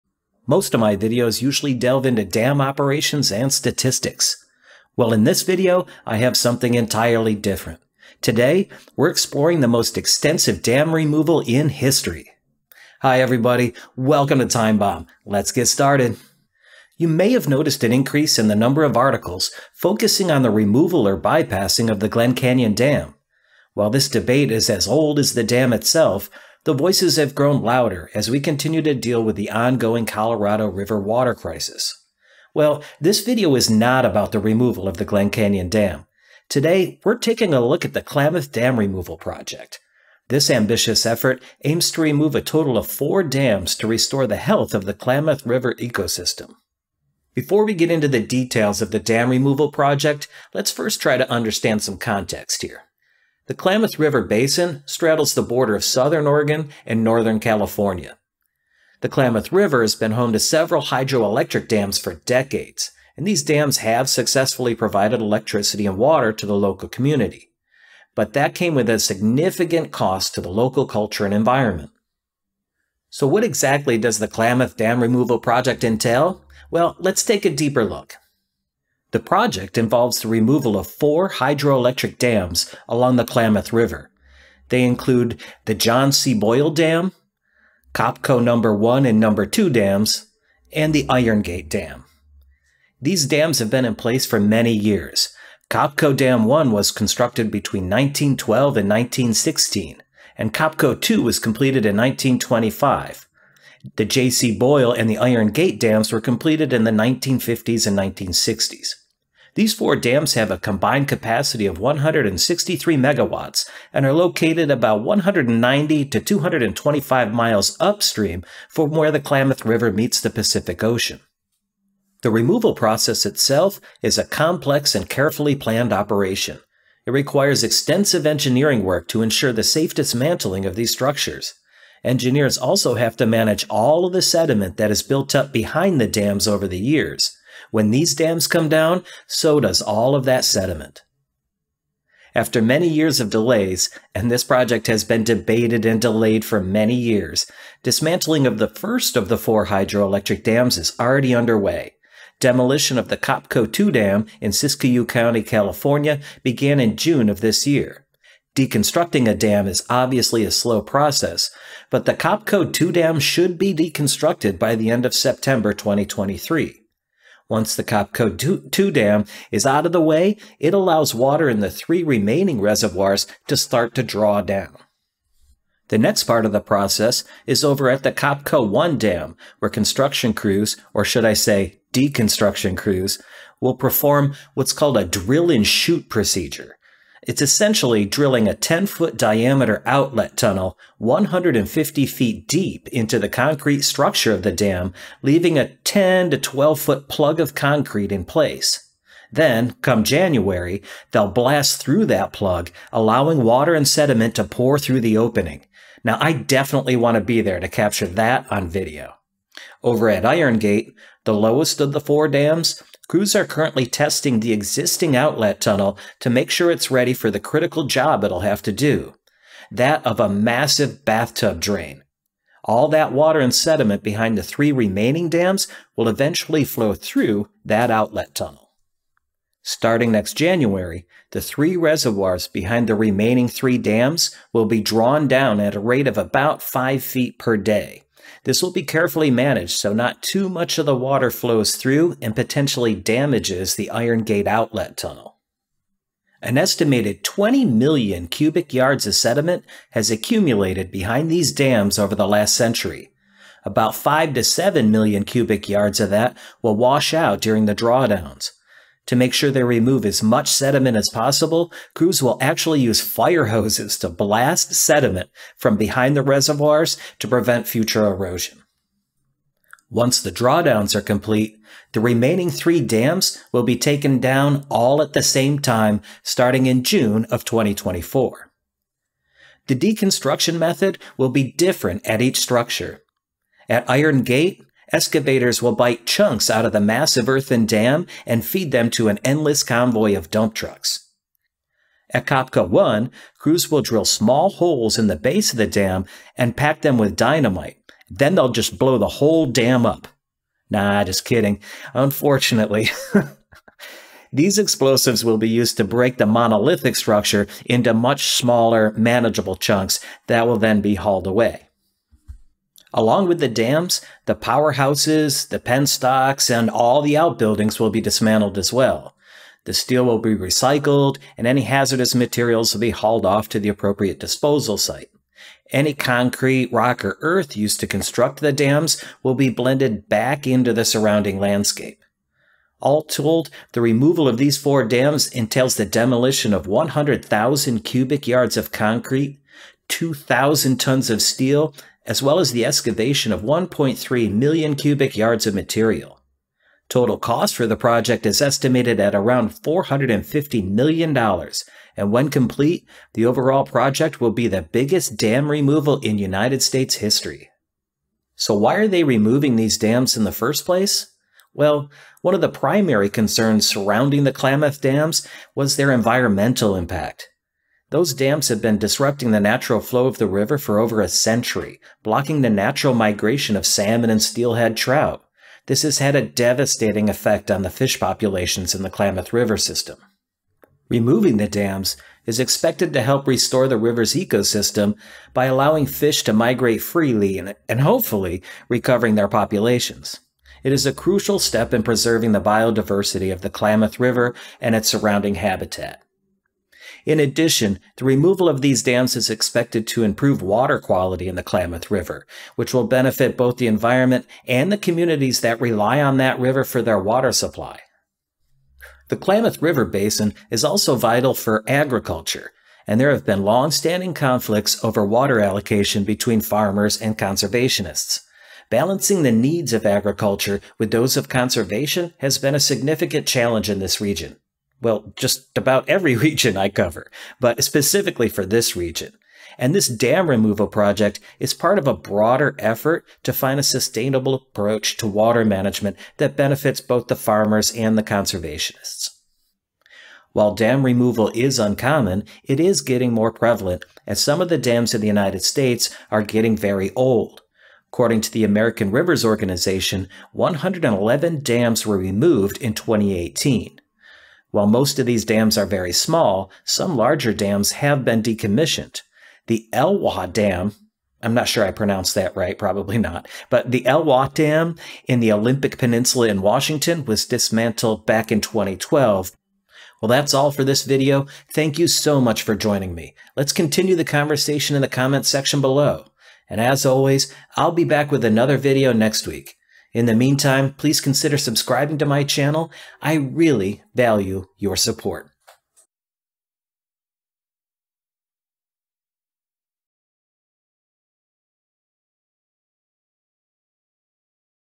The World's Largest Dam Removal Project
Welcome to Time Bomb! In this video, we embark on a journey into one of the most monumental environmental endeavors of our era - the Klamath Dam removal project. While our channel typically delves into the intricacies of dam operations and statistics, today, we venture down a distinct path, one that's dedicated to restoring the delicate balance of nature.

The Klamath Dam Removal project sets out to liberate the Klamath River from the constraints of four imposing dams. In this video, our primary focus centers on the intricate engineering marvels that underpin this colossal undertaking. We'll uncover the meticulous plans for safely bringing down these formidable structures, leaving no stone unturned in our quest for knowledge.

But that's not all; we'll also explore the post-dam removal landscape, delving into what unfolds once these barriers are dismantled, and the staggering financial commitments tied to this ambitious venture.

This is the epic tale of the grandest dam removal project in the annals of U.S. history. It's a story you won't want to miss!"